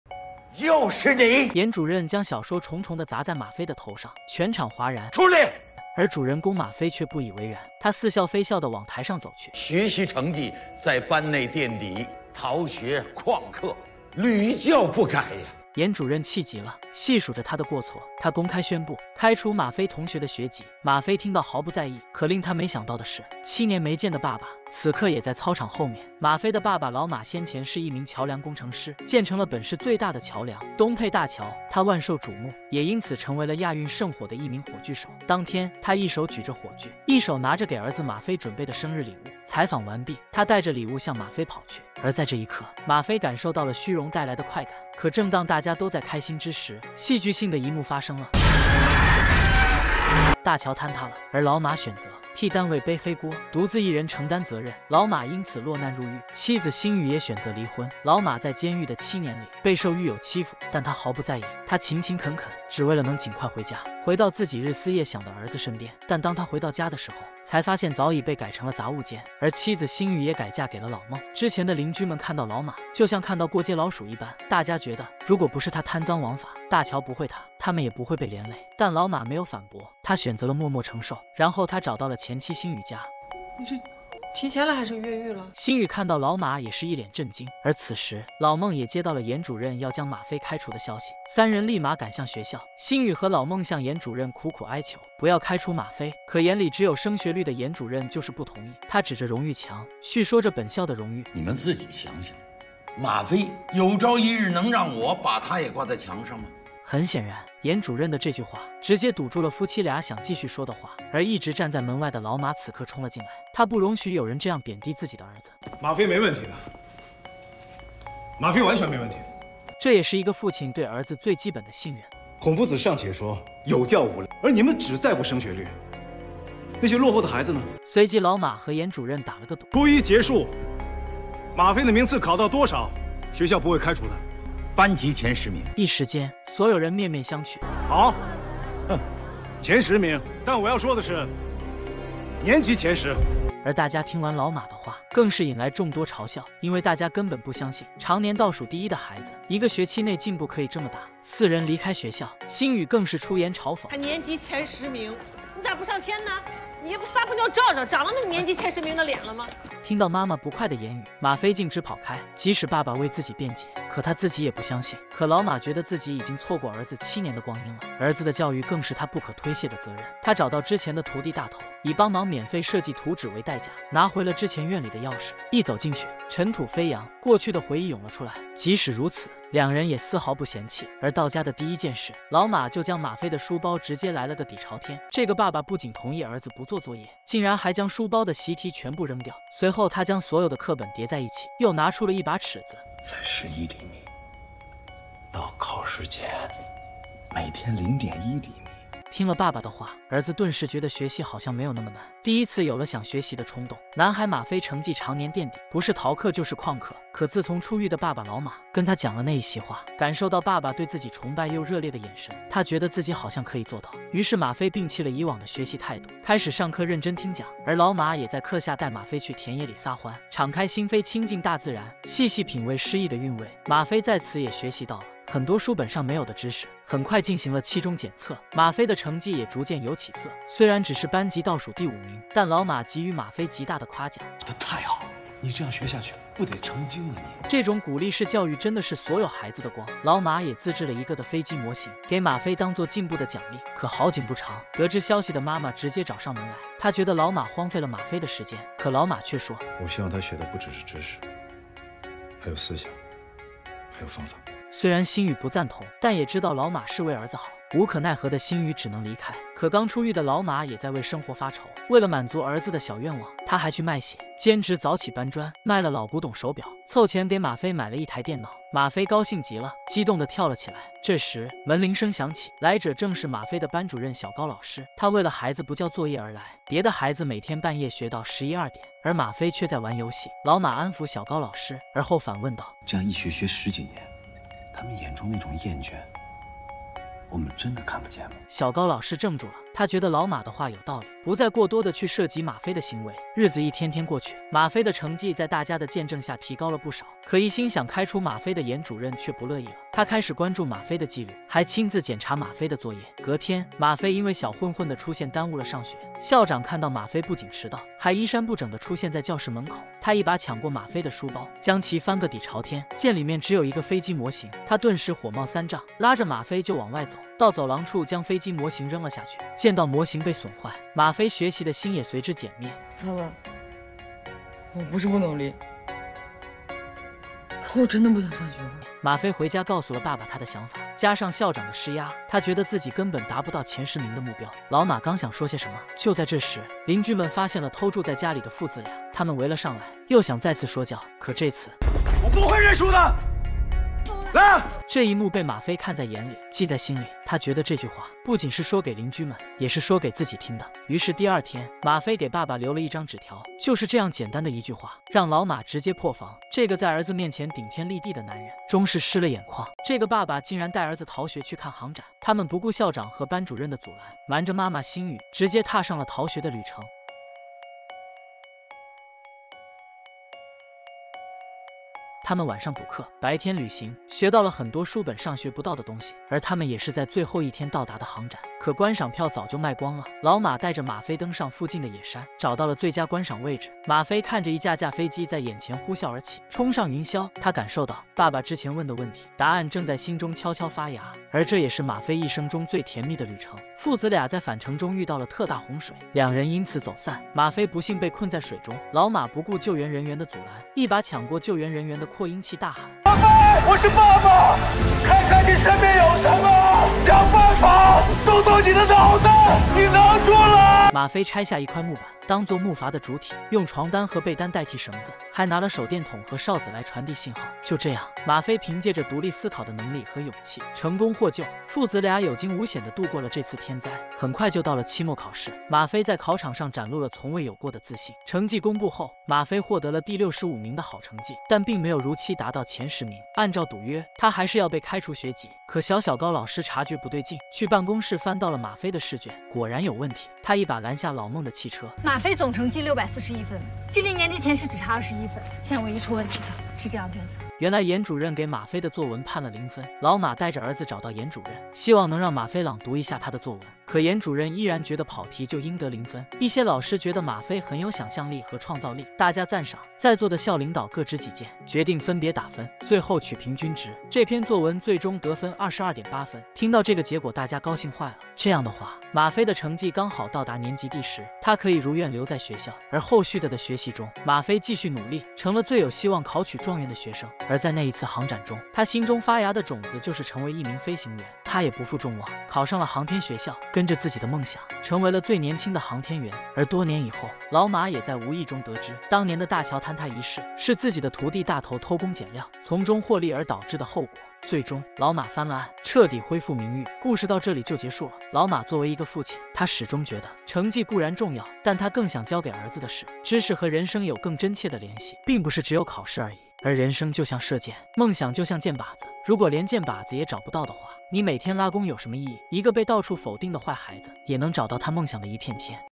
出獄爸爸教育問題少年，倒數第一逆襲年級前十，過程卻令人淚目
本片簡介：
浩瀚太空，太空人意外失聯，生命最大的絕境中，他回憶起自己那個最了不起的爸爸。一對父子跨越漫長的時光，守護愛和親情，故事充滿了歡樂、溫暖、淚水與奇觀。